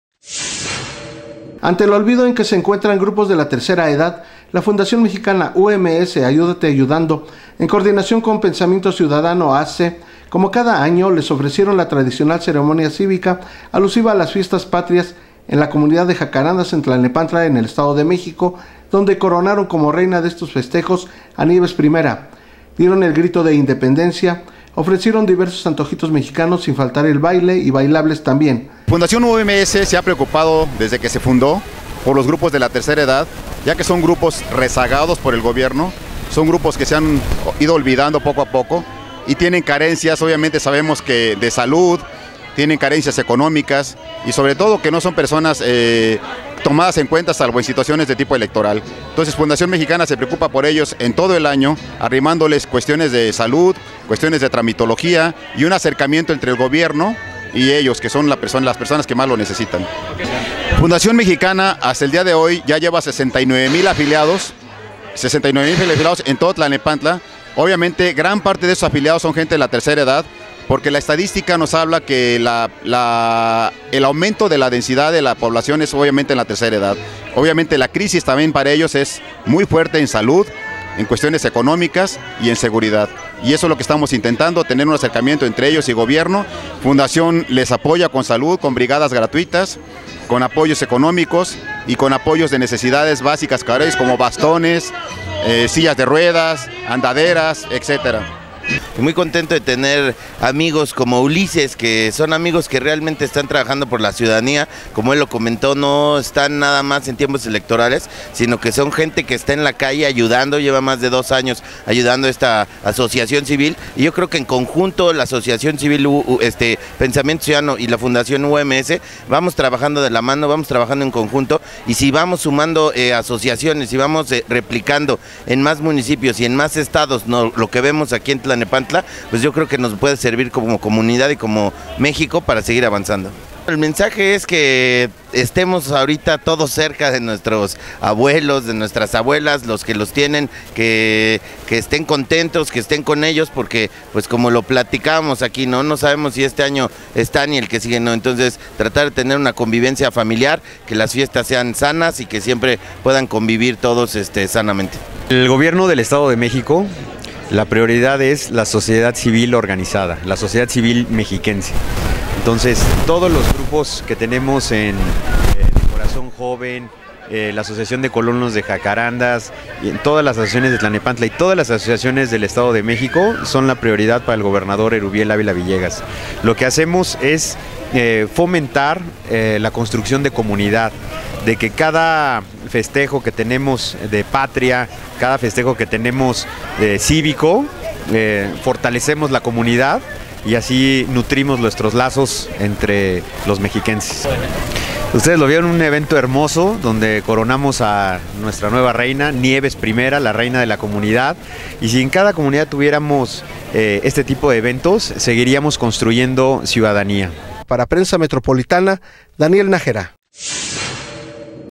Festejo patrio con los adultos de la tercera edad
Se llevo acabo en jacarandas tlalnepantla el festejo patrio con adultos mayores.